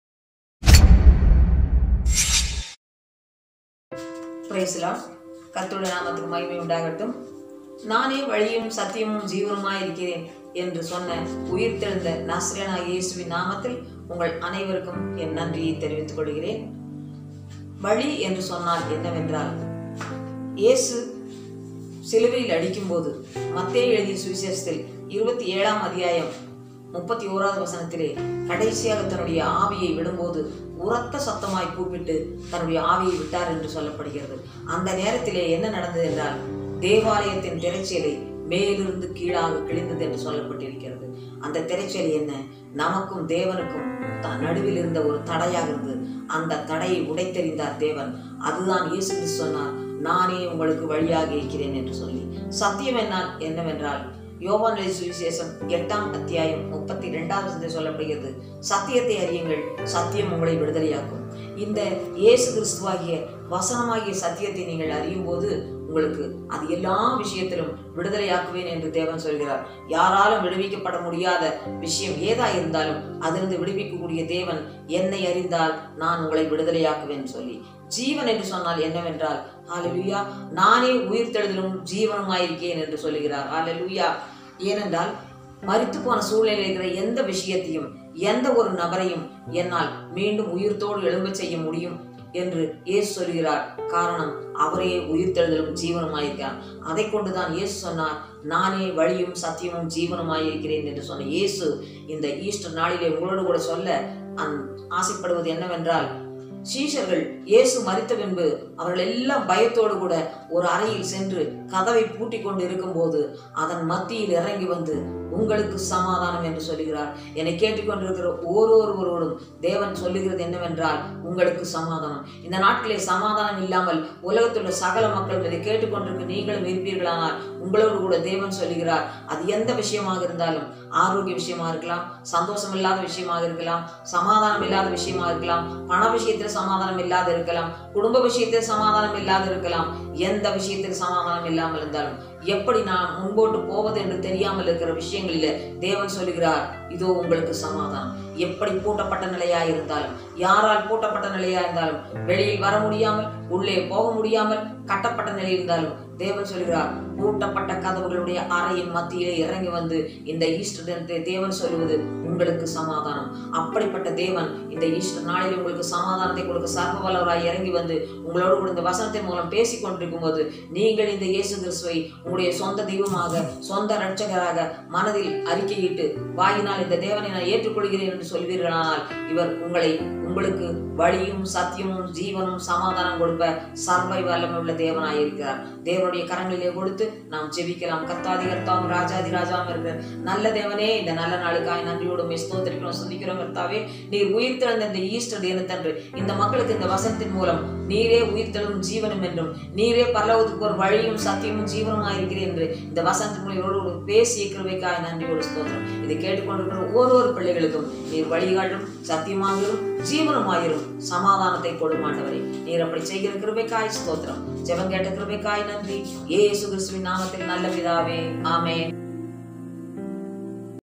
🔴✝Jesus Christ Gospel Video Easter Special [Tamil] John 14:6 | John 8:32
✝Jesus Christ Gospel Video John, Chapter 14

6. Jesus answered, "I am the way and the truth and the life. No one comes to the Father except through me.

6. அதற்கு இயேசு: நானே வழியும் சத்தியமும் ஜீவனுமாயிருக்கிறேன்; என்னாலேயல்லாமல் ஒருவனும் பிதாவினிடத்தில் வரான்.